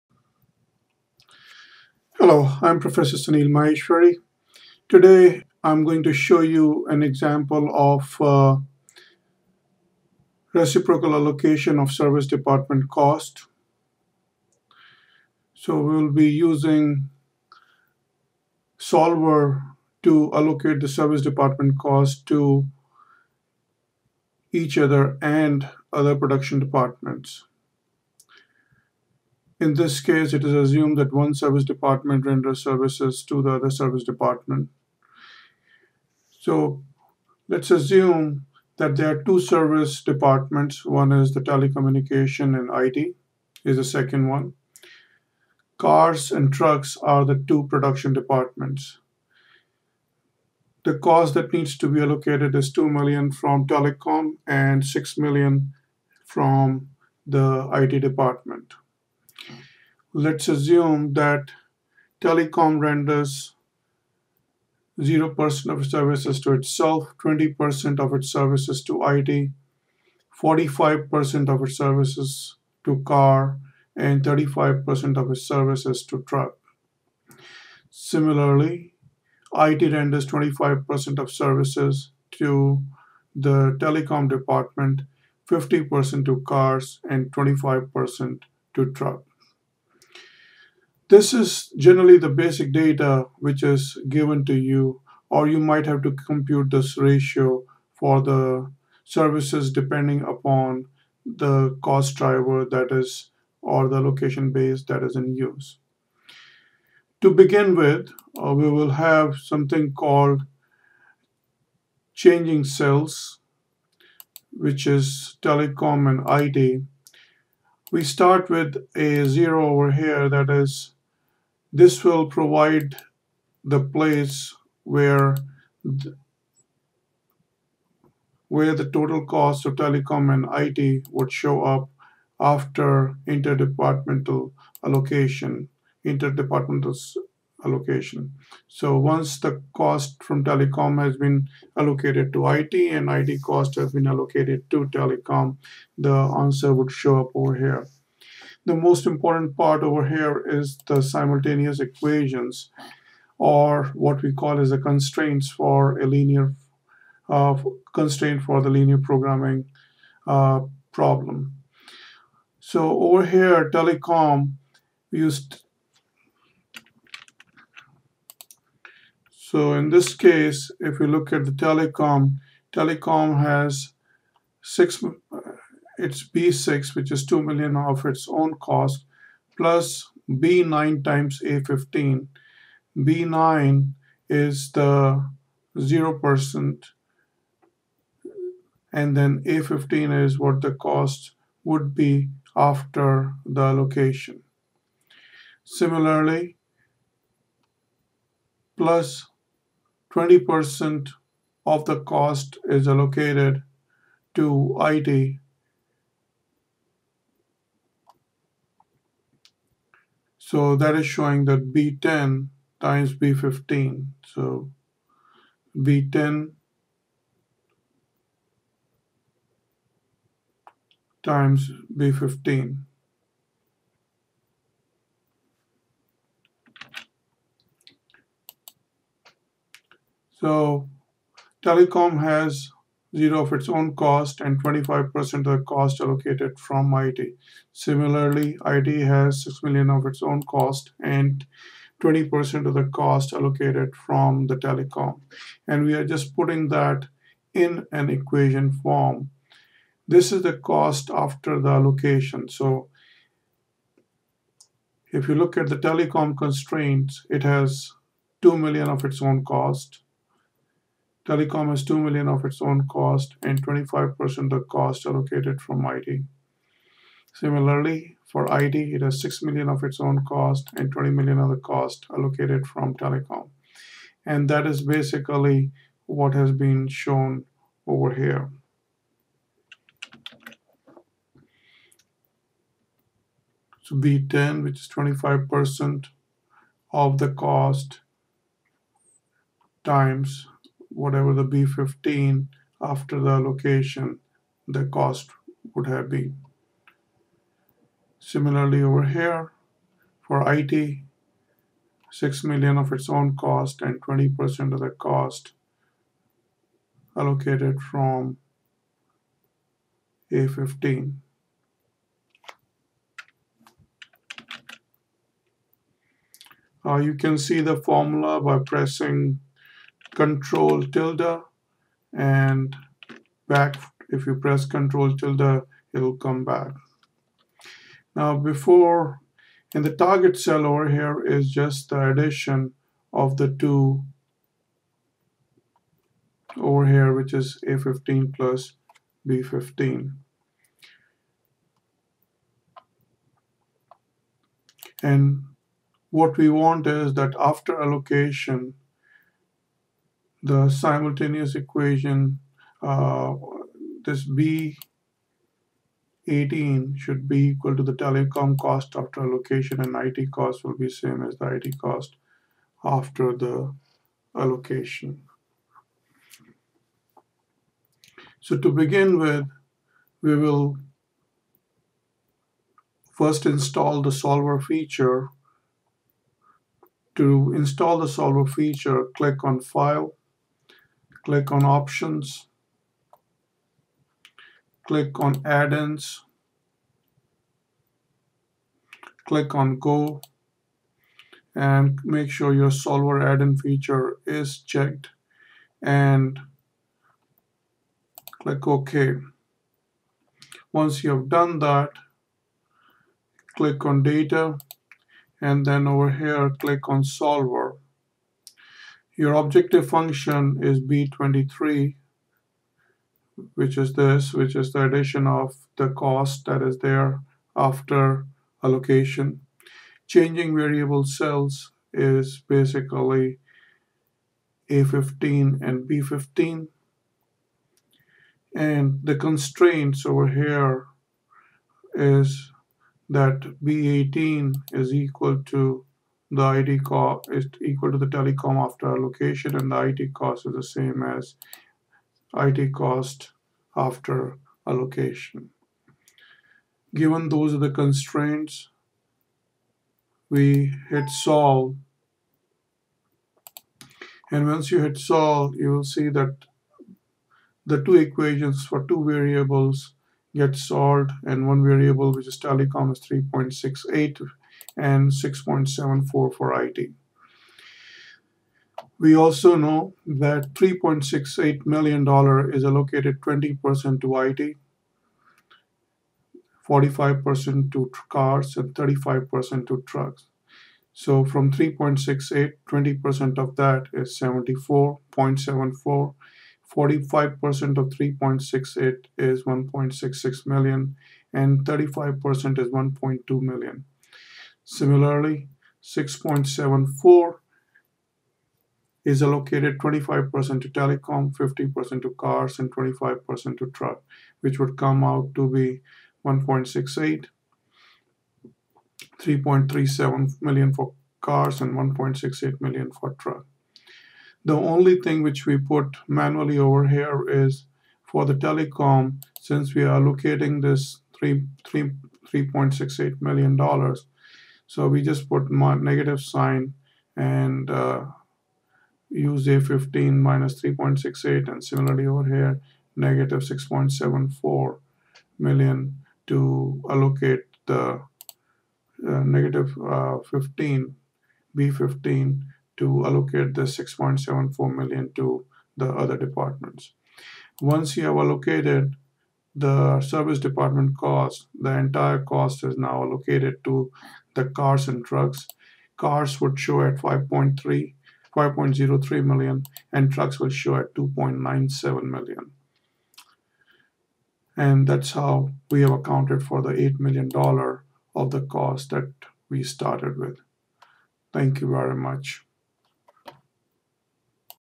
Reciprocal Allocation of two Service Dept cost using Solver as per book
This is an example to show steps involved in using Excel Solver for reciprocal allocation of service dept. costs. This is part of a class.